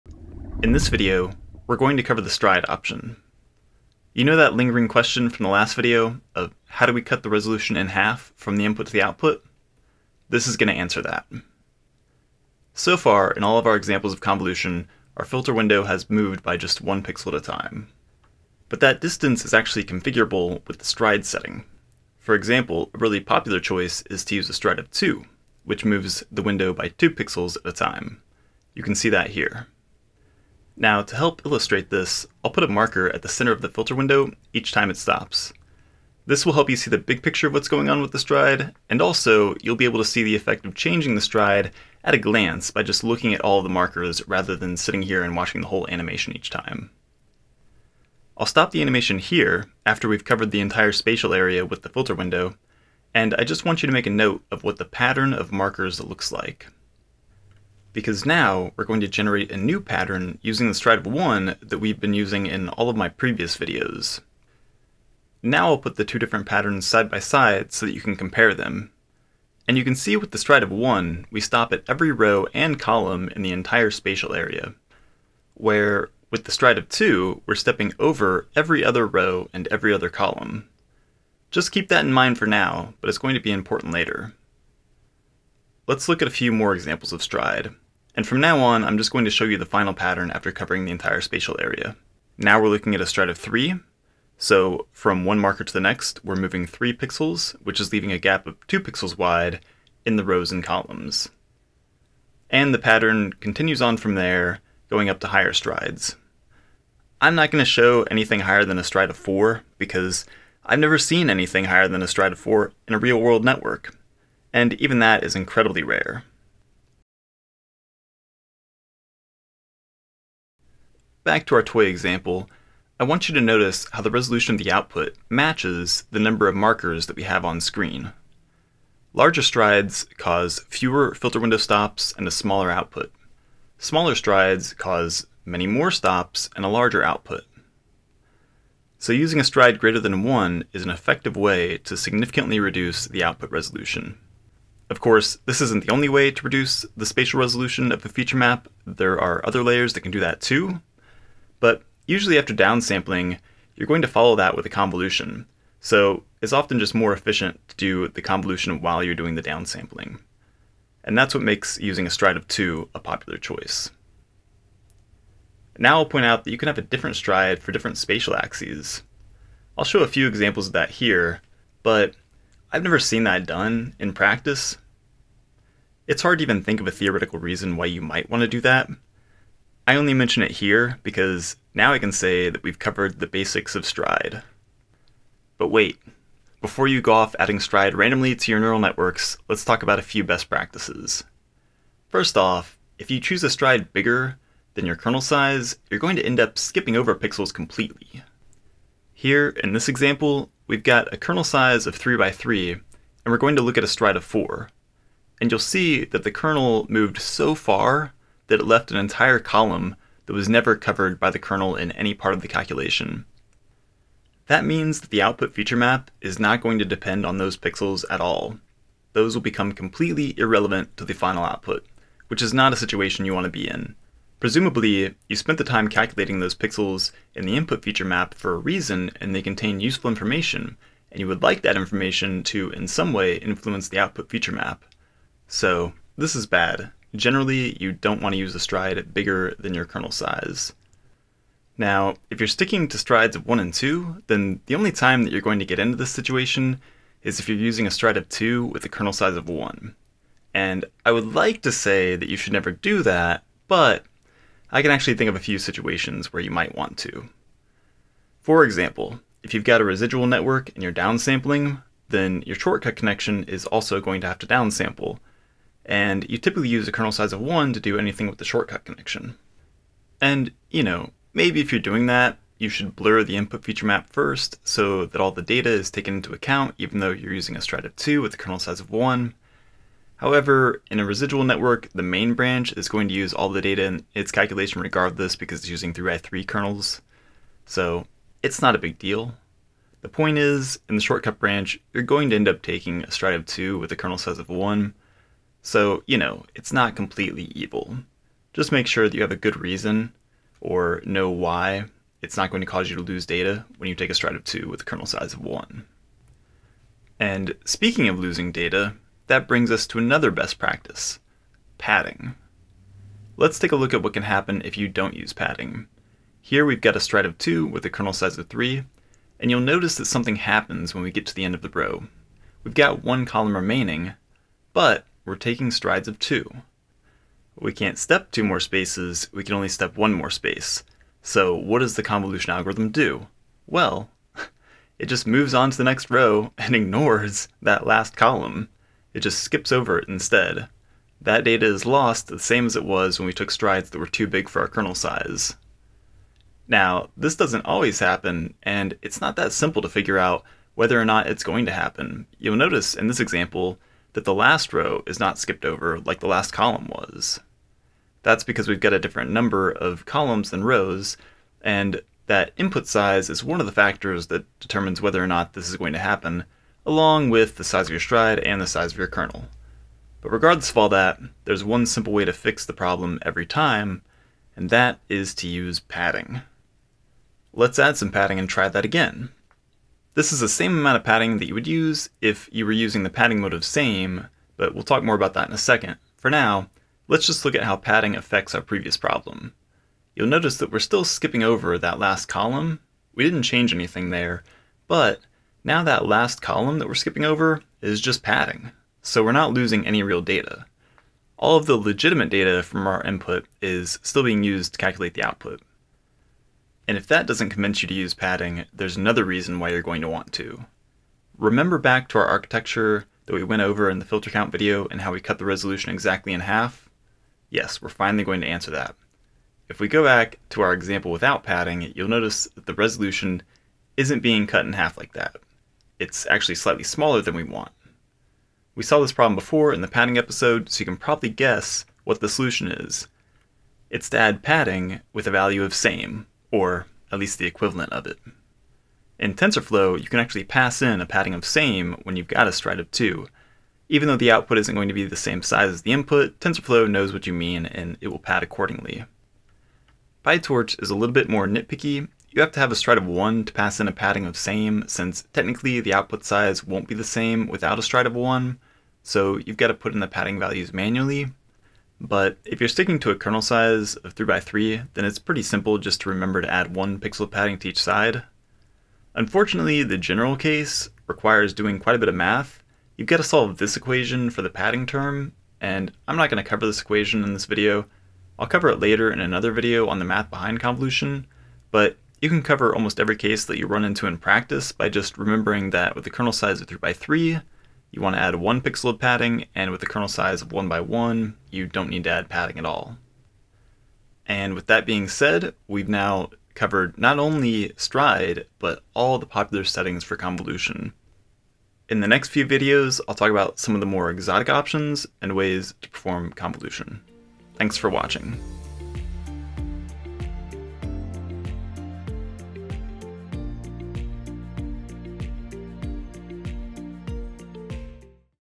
Stride - Convolution in Neural Networks
A brief introduction to the stride option in neural network convolution followed by some best practices.